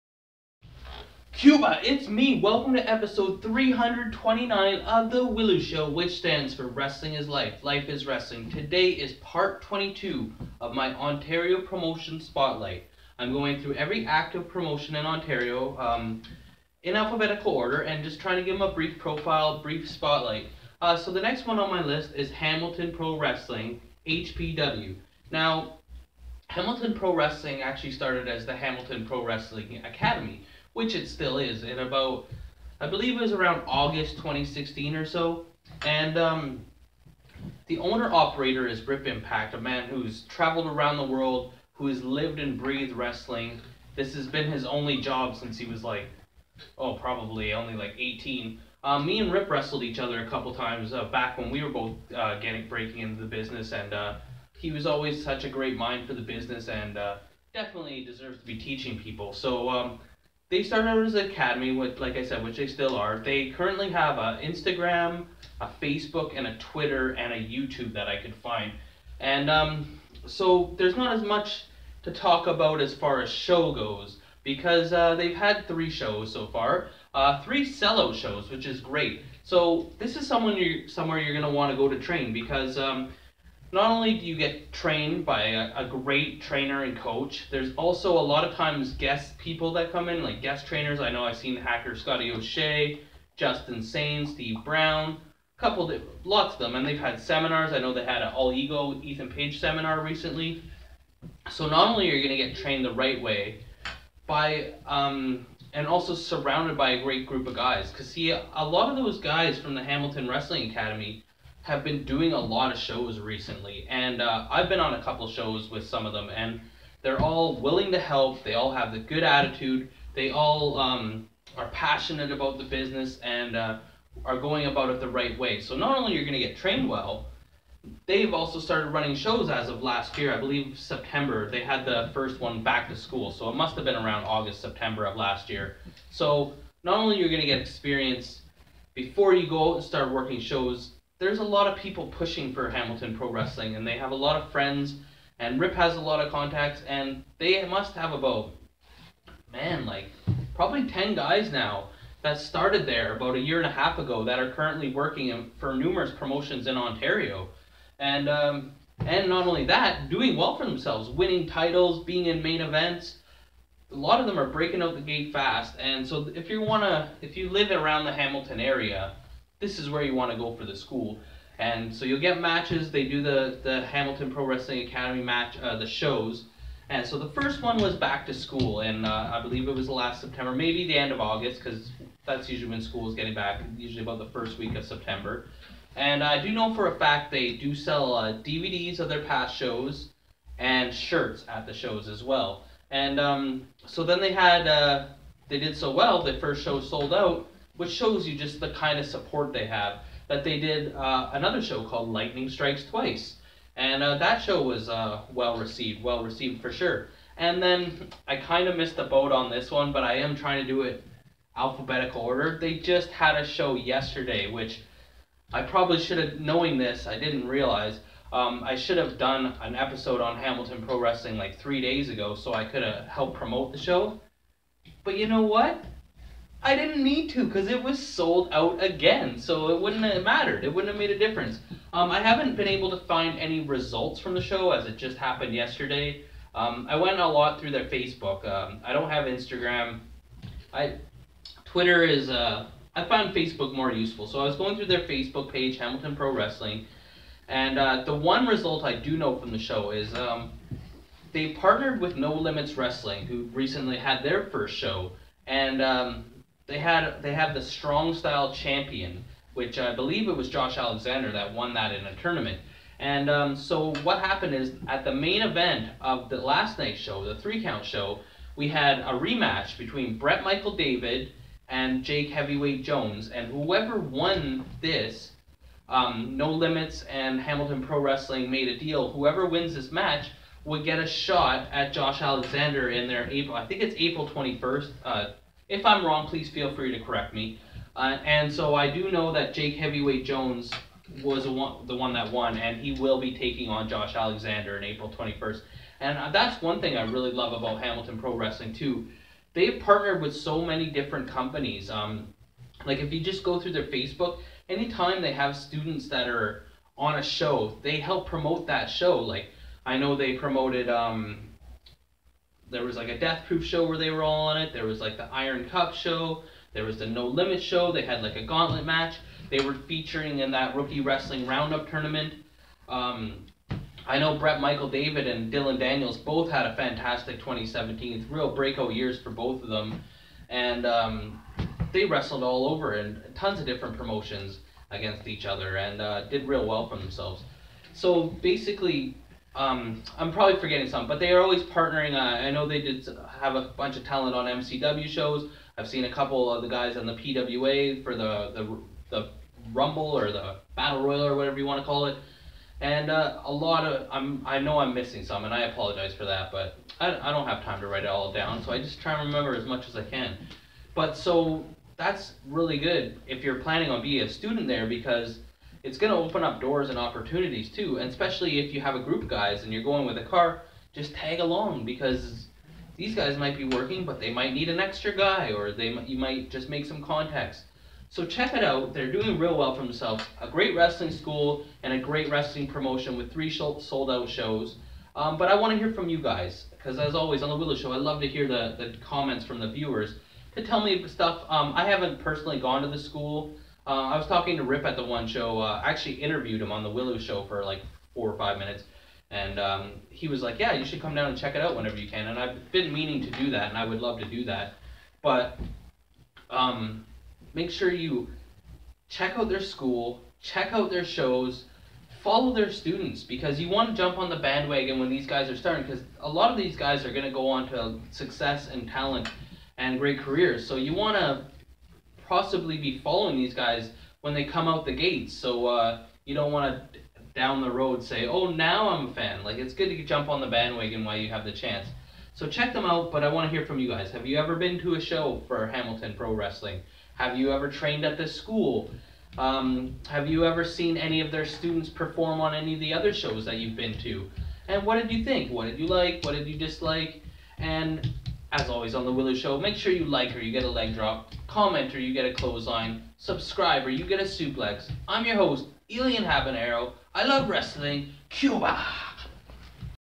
W.I.L.L.I.W. Episode 329 - Promotion Spotlight (Part 22 - Hamilton Pro Wrestling)
W.I.L.L.I.W. Stands for Wrestling is Life, Life is Wrestling. Today Elian Habanero profiles Hamilton Pro Wrestling on his Ontario Promotion Spotlight Series.